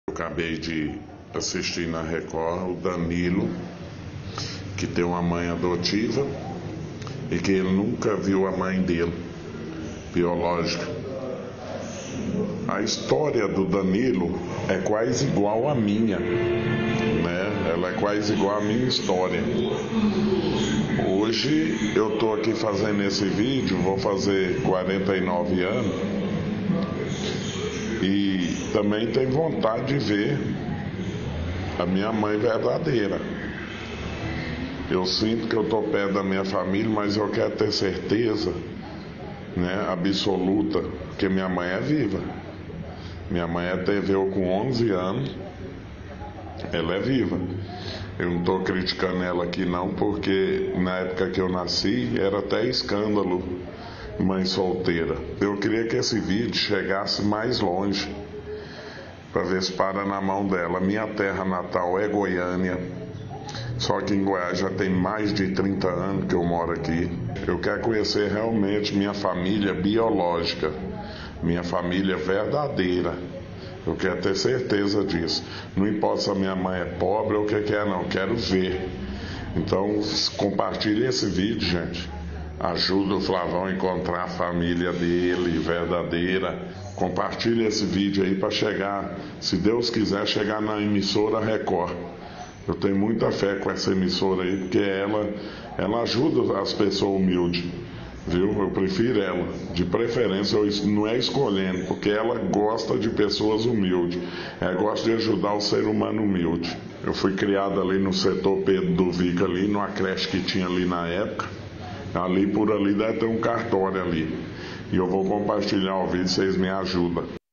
HOMEM QUER AJUDA PRA ENCONTRAR A MÃE
O publicitário Flavão, morador da cidade de Goiás, compartilhou um vídeo nas redes sociais com a esperança de que ele chegasse até aqui na RecordTV para que ele pudesse encontrar a mãe biológica. Vou te mostrar agora um pouquinho da história dele.